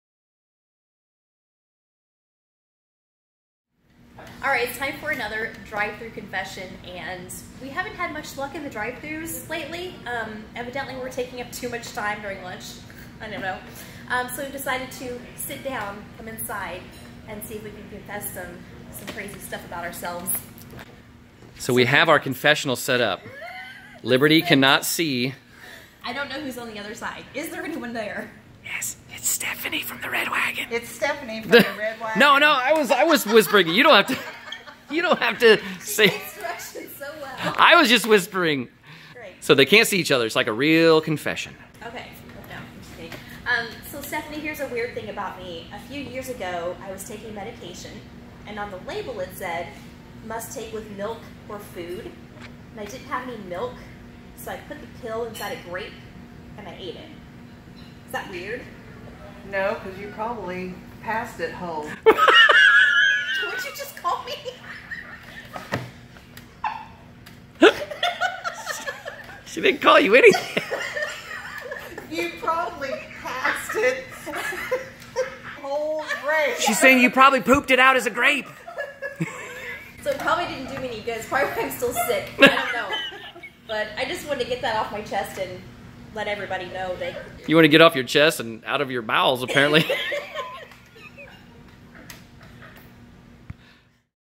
Drive Thru Confession | Liberty at the Red Wagon
Liberty sat down in our make-shift confessional at the Red Wagon in Poseyville to get another weird confession off her chest.